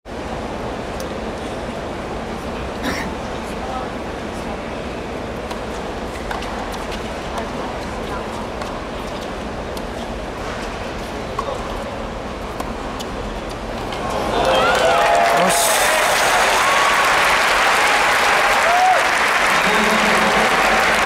US OPEN 2018 SF Nishikori vs Cilic
2018/9/6 Arthur Ashe Stadium @Flashing Meadow, NYC